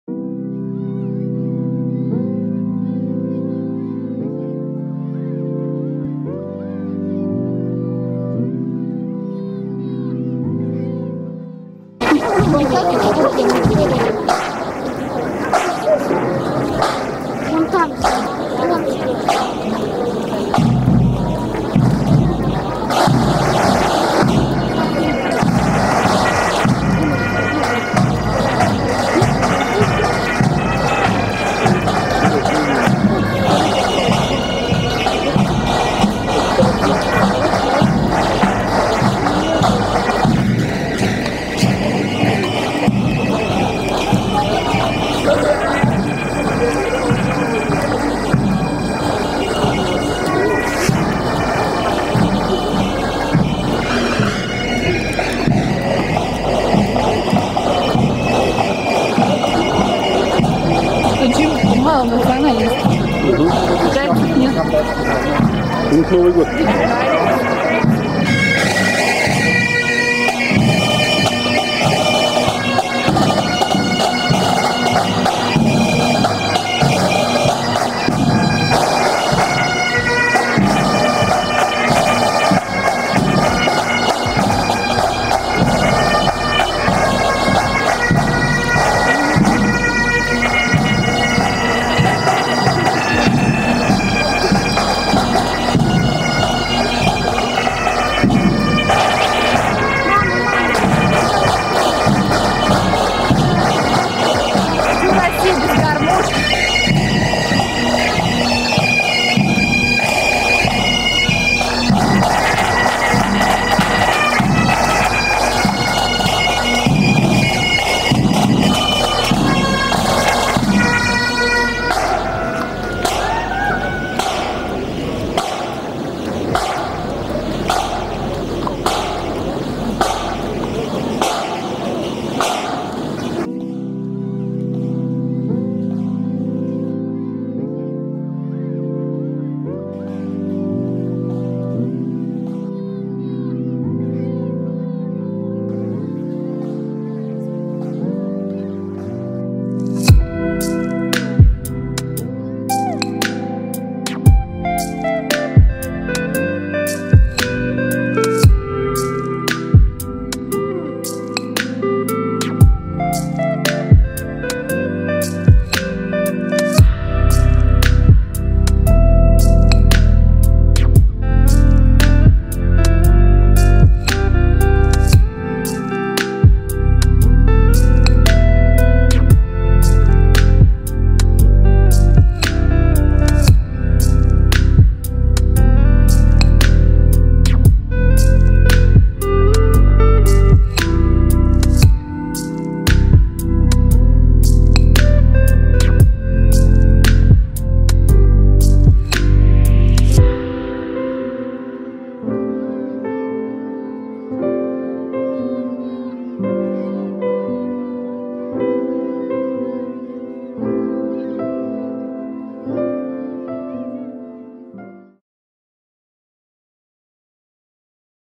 Русская община на Фестивале 1997 года
9 февраля 1997. Русская община на параде национальных общин.

Первый фестиваль национальных культур в Канберре состоялся в 1981 году и проходил в день Австралии.  К 1988 году, фестиваль "вырос" до сорока палаток с кухней разных народов мира, музыкой и танцами. Мероприятие проводилось в центре Цивика на площади и Эйнсли-Авеню. Был парад участников в национальных костюмах во главе с китайскими танцующими драконом и львами.

В 1997 году было решено проводить фестиваль в течение целой недели. Этот год считается официальным годом рождения Всеавстралийского фестиваля национальных культур.

 Помимо участия в параде в 1997 году еще была палатка русской кухни. Продавали только пирожки, которые пекли члены православного прихода церкви.